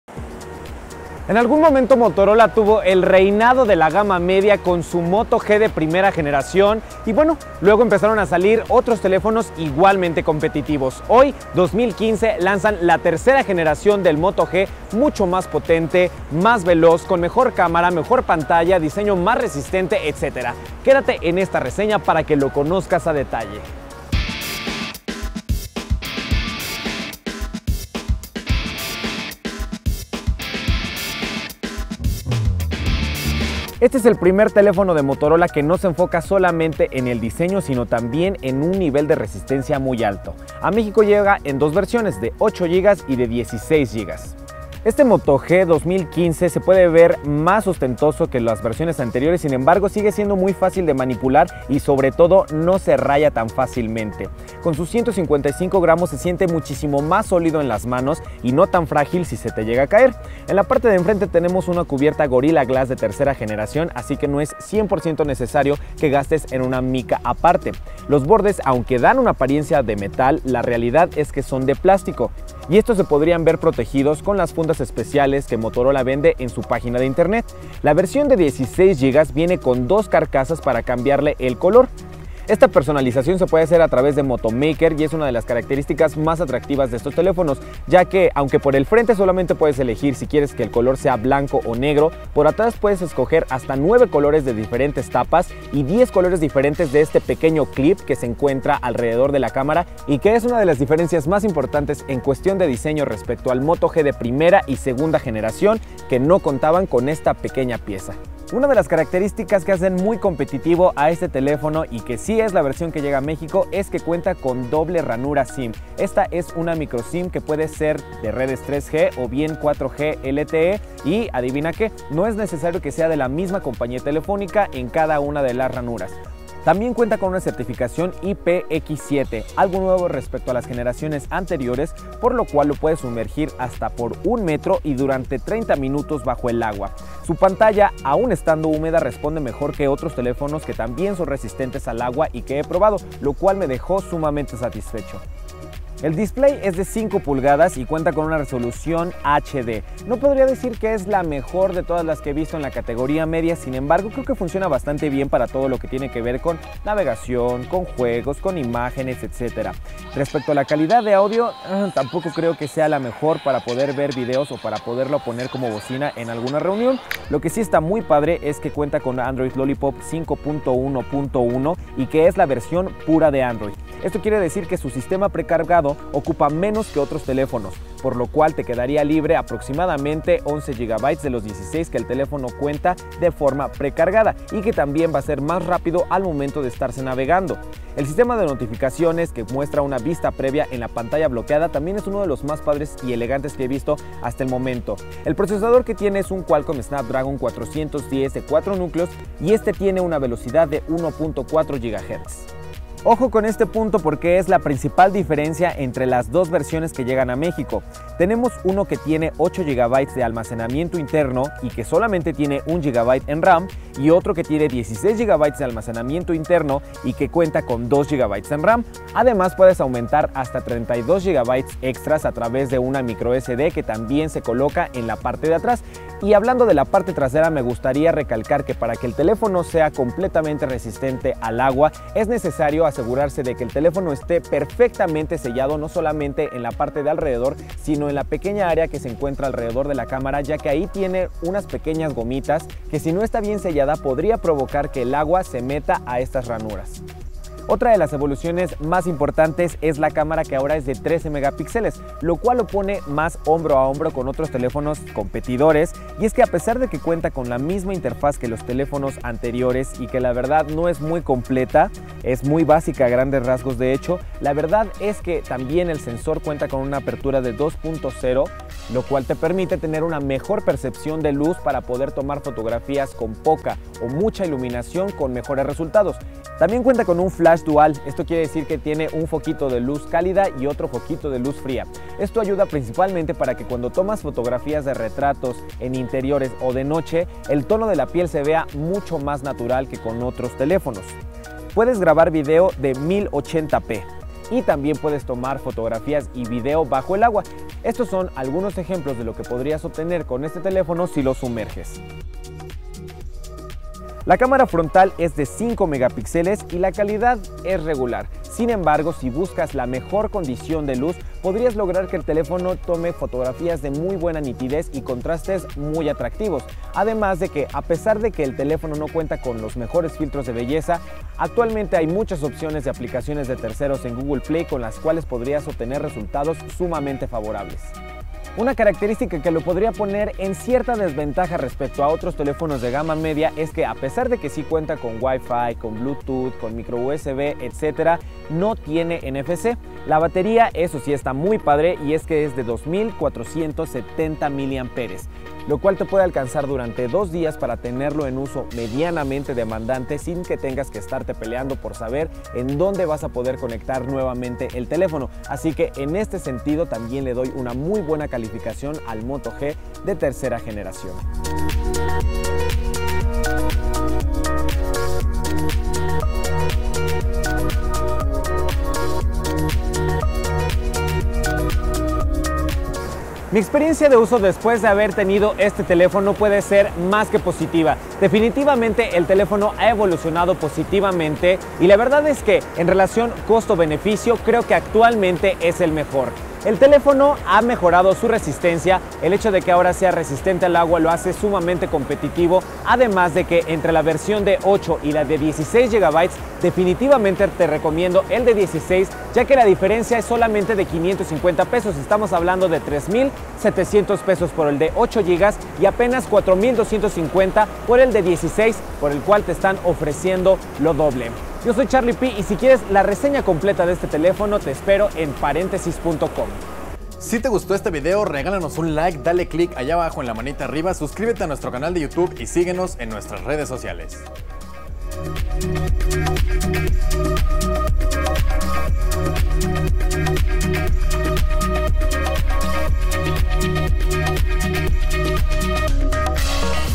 Motorola Moto G 3a generación, review en español
El Moto G 2015 ha regresado a pelear el reino de la gama media. Para ello llega con un cuerpo mucho más resistente que nunca capaz de sumergirse en agua y equipado con una cámara de 13 megapixeles que podría hacerte dejar tu cámara compacta en casa.